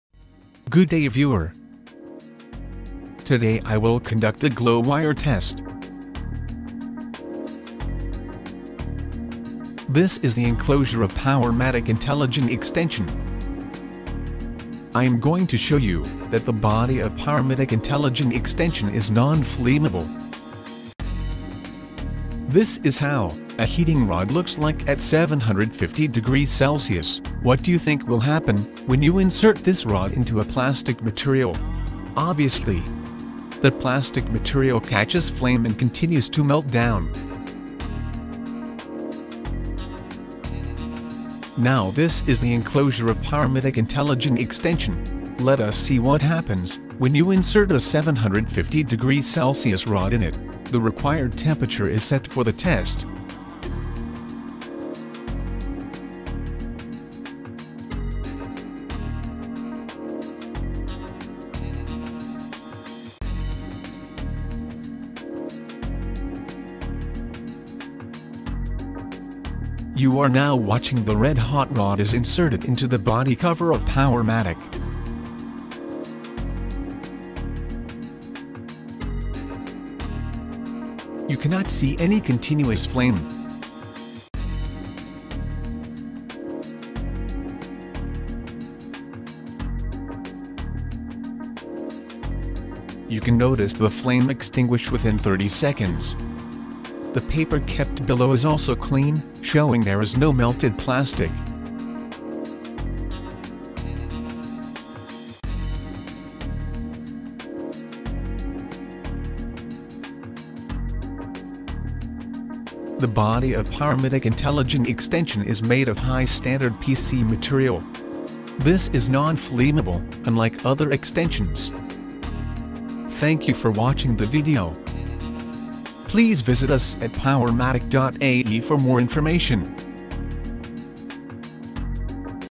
Powermatic - Glow-wire Flammability Test
Powermatic Intelligent Extension goes through various test to ensure its quality of every batch, and assures that all the material being used is of high quality. This test is one of the many test and it measures if the body being tested is non-flammable, as it lets a 750C rod enter the body of the extension.

According to; IEC Glow-wire flammability index GWFI
In accordance with IEC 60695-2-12 standards, the GWFI is expressed as the highest test temperature during three subsequent tests for a test specimen of a given thickness, at which one of the following conditions is met;
1. Flames or glowing of the test specimen extinguish within 30 s after removal of the glow-wire, and there is no ignition of the wrapping tissue placed underneath the test specimen.
2. There is no ignition of the test specimen.